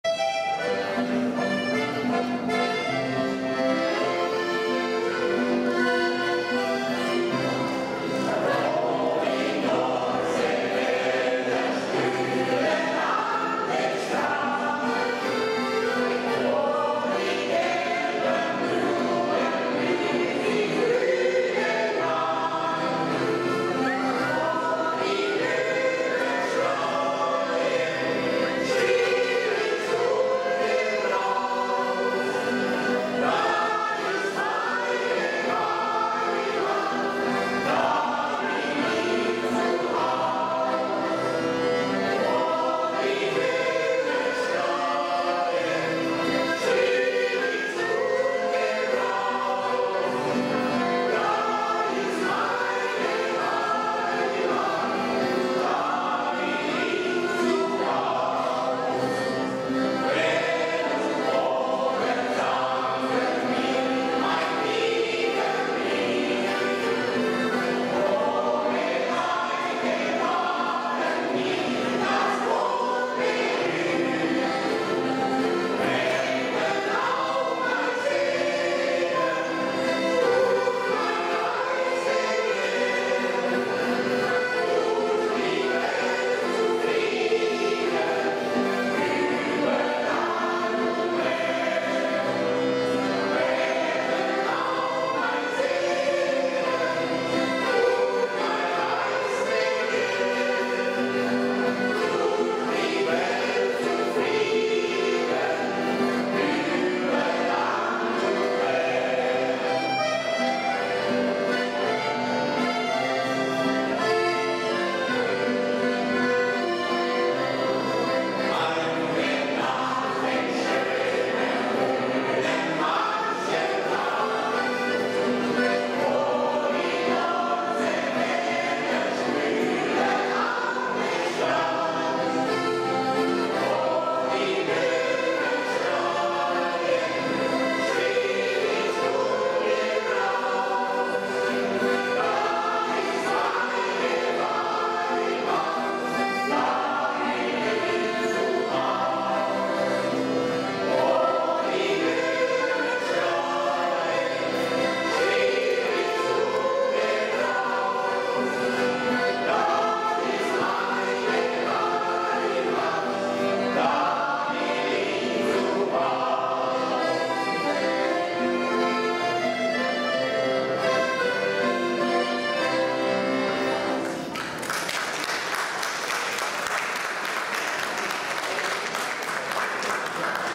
2018 Deutsches Forum Bacau
Jahrestreffen der deutschen Minderheit im Judet Bacau und Vorstandswahlen